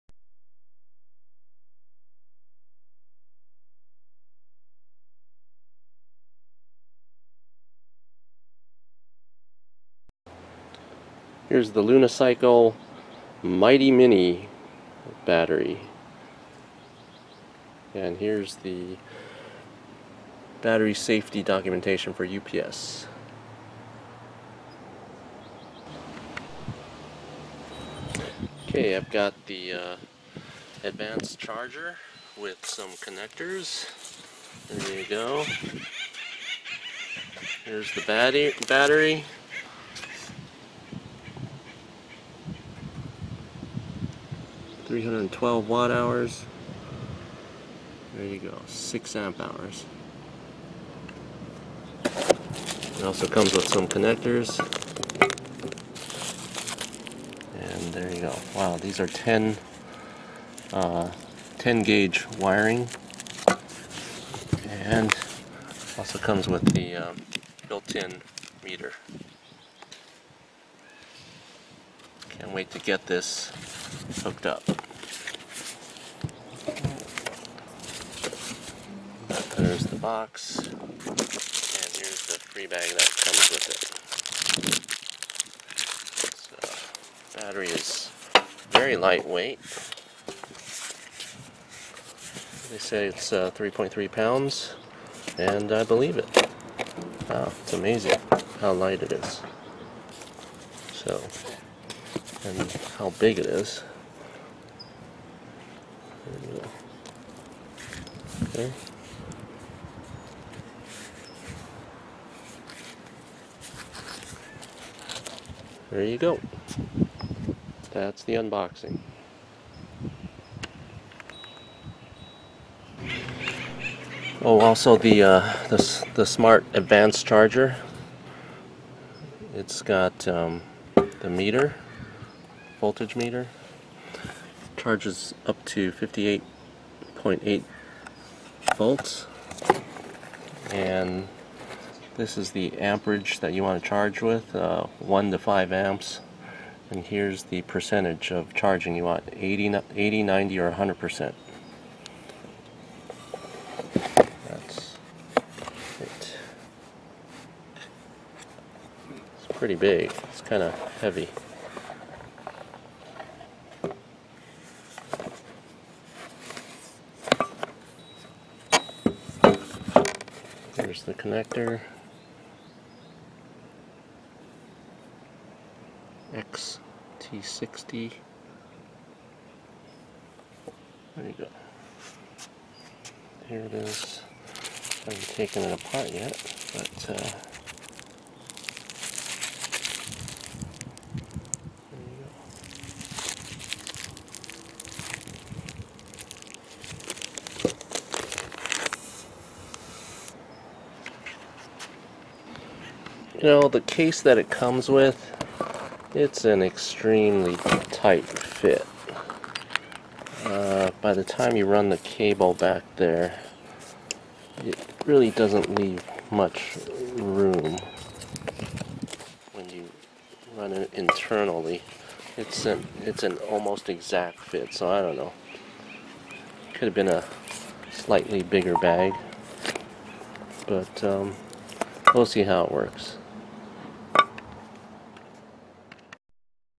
LunaCycle 52v Mighty Mini Battery and Advanced Charger
Update June 2020 - I've only got 300 miles on it but it only charges to 56v instead of 58.8v. Admittedly, it was dormant for a long period of time and stored outdoors (under cover). I have a feeling a cell is going bad. I also dislike it when the power shutdown in a low voltage situation - this isn't good because you need to "jumpstart" it to reset it. Sometimes you are pushing the bike for a second or two and the power cuts off even though there's some capacity left in the battery - you just can't push it, you need to go slow and steady.
Update Dec 2017 - Range sucks when you go high speeding and uphills. But when you go slow and constant (ie 10 mph on flat surface, you can do 20 to 30 miles).
Update Oct 2017 - on my 48v 1000watt hub motor, I'm getting about 7 mile range. Kind of disappointed with the range, but I needed something that small and light to fit in a full suspension the bottle cage area.
LunaCycle.com 52v 6ah Mighty Mini Battery ($300)
 and Advanced Charger ($80)
Battery is around 3 pounds.  6" x 3.75" x 3.25".
Samsung 30q 18650 Batteries and BMS.
Luna Advanced Smart Charger charges from 1a to 5a. Also charges 80%, 90%, or 100% of battery capacity. The lower you charge, the more cycles the battery can be charged.
With voltage display.

This battery is pretty awesome. My bike won't fit a regular dolphin, shark, bottle, etc battery pack. This is small enough to fit. At 312 watts, it should be good for 15 miles fully charged (your mileage may vary). They have a 7ah version with Panasonic batteries and slightly better range. Unfortunately, they were out of stock, so I ended up getting this - which suppose to have somewhat better performance at high power usage. For me, I wanted better range - oh well. Either way, it is quite expensive.

The Advanced charger is quite interesting. It's got multiple settings and a voltage display. It would have been great if it was a lighter and smaller - which is my only complaint. Perhaps the mini smart charger would have been a better fit for me - but it doesn't have a voltage display.